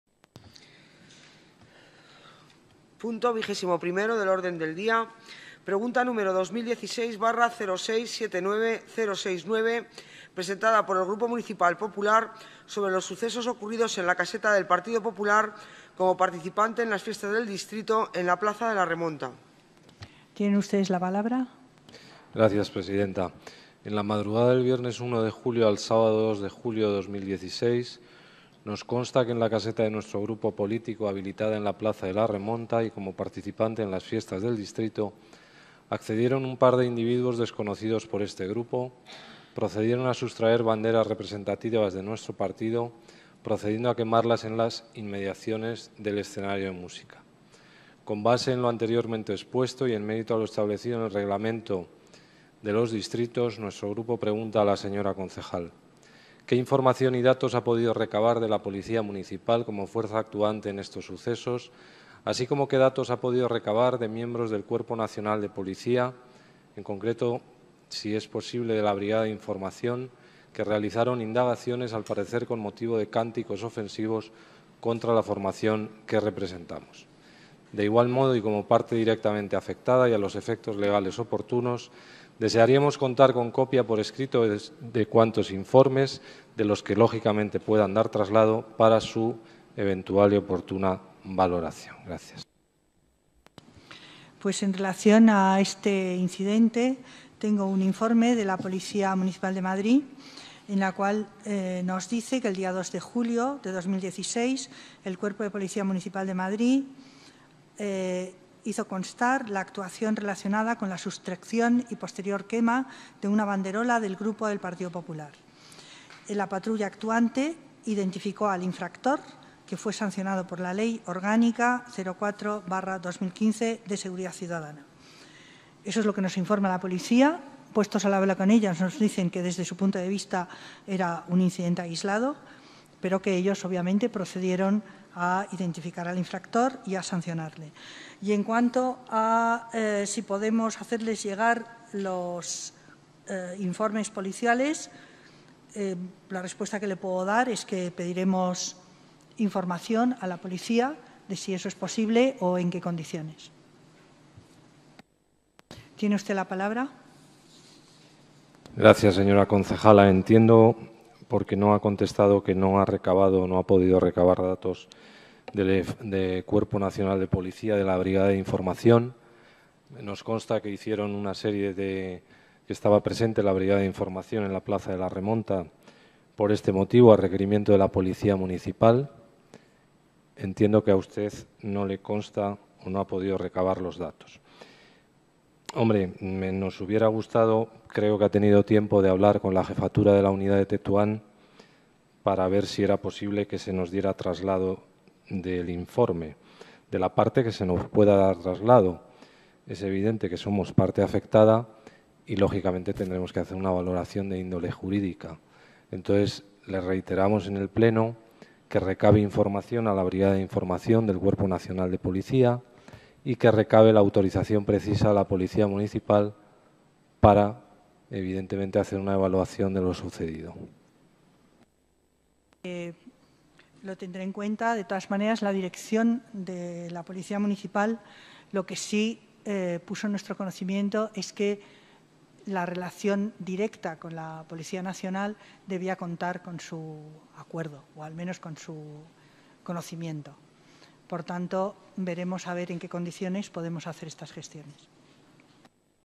Pregunta de Manuel Gómez sobre sucesos en caseta PP fiestas
Pregunta de nuestro portavoz Manuel Gómez sobre los sucesos acaecidos en la caseta del PP en las fiestas del Distrito.